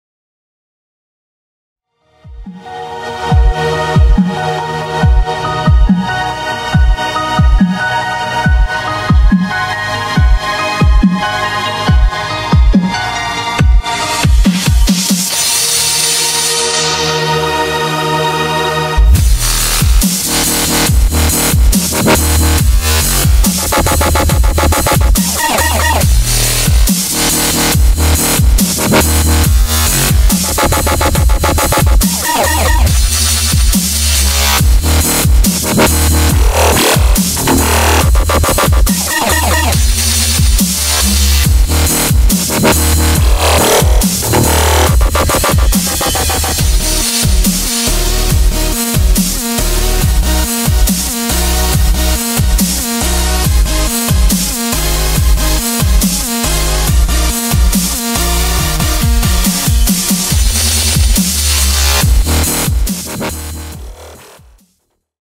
Whatsapp Circles by xkyy 100% (FIRST VICTOR?)
it was a lvl race with @gekagd_ , video soon on his chanel.(i post this as a first victor only on yt btw)

About level
Level name: Whatsapp circles
Creator: xkyy
Difficulty: 3/10 (imo high tier hard demon)
Attempts: 518
Enjoyment: 75/100 (has a lot of choke points)

Info
In game nickname: pipsok
fps/hz: 240hz and Vertical Sync